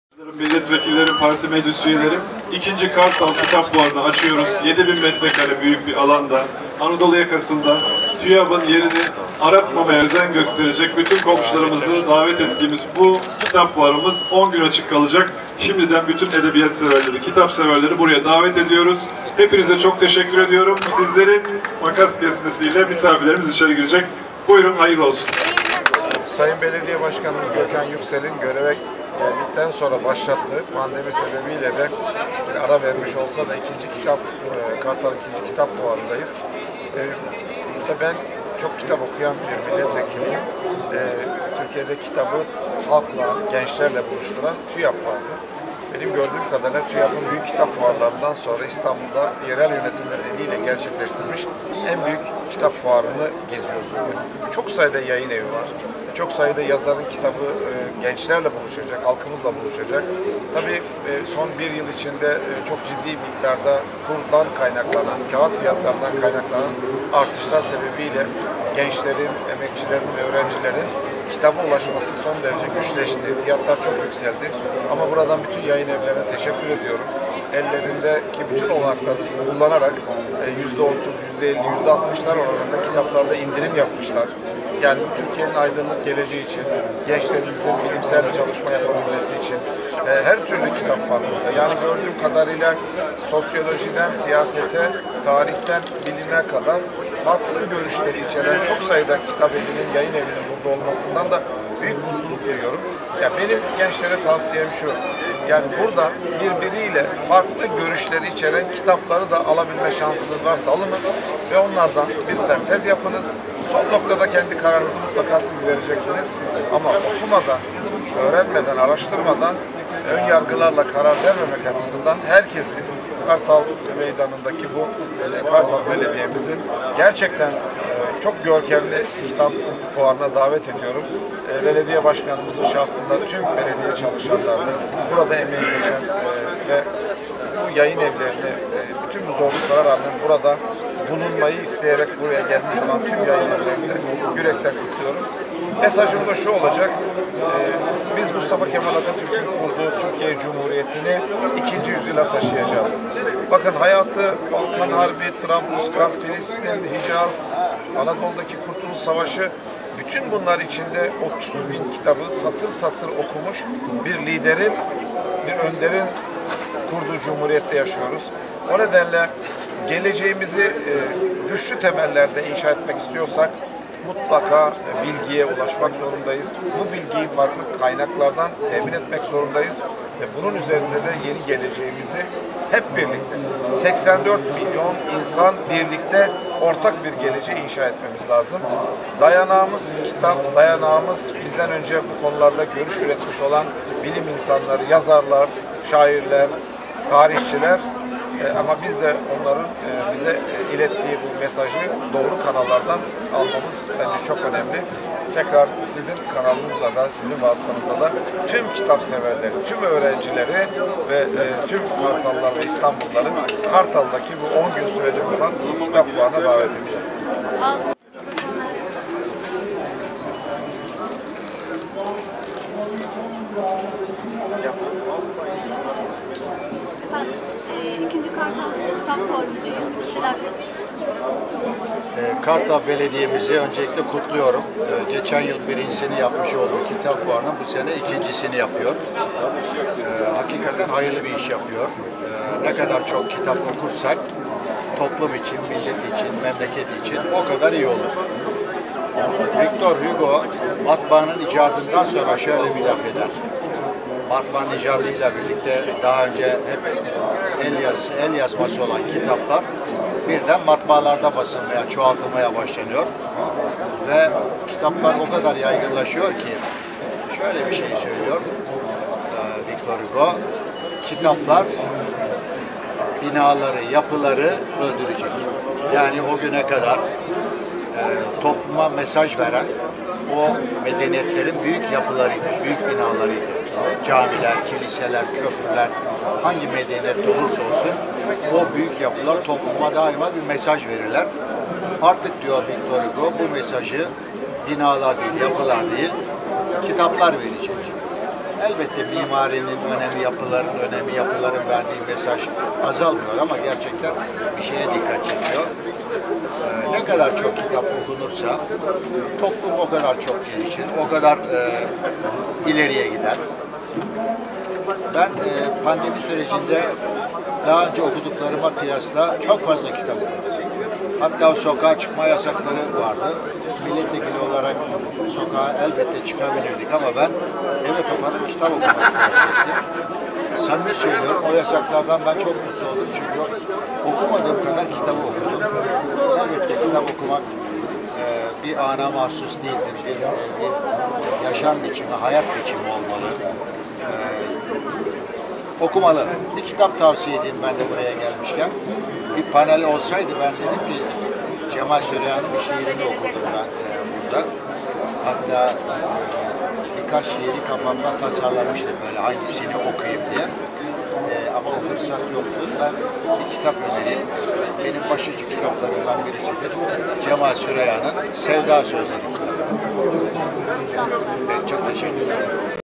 2. Kartal Fuarı Açıldı
CHP Milletvekili Gökhan Zeybek ve CHP Milletvekili Mehmet Akit Hamzaçebi Kitap Fuarı'nda kitabın önemi üzerine mesajlar verdiler.